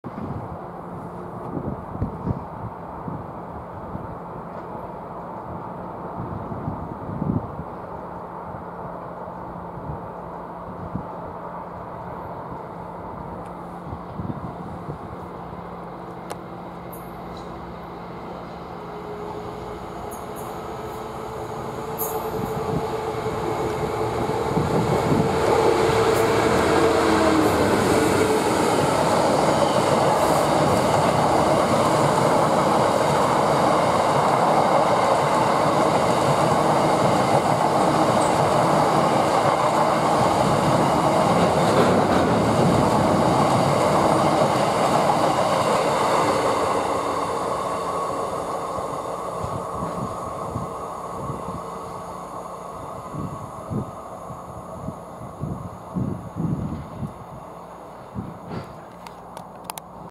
DB schenker 66200 with Toton north yard to East usk jn empty stone train
66200 passes through Severn tunnel junction with Toton north yard to East usk jn empty stone train. 10/9/2015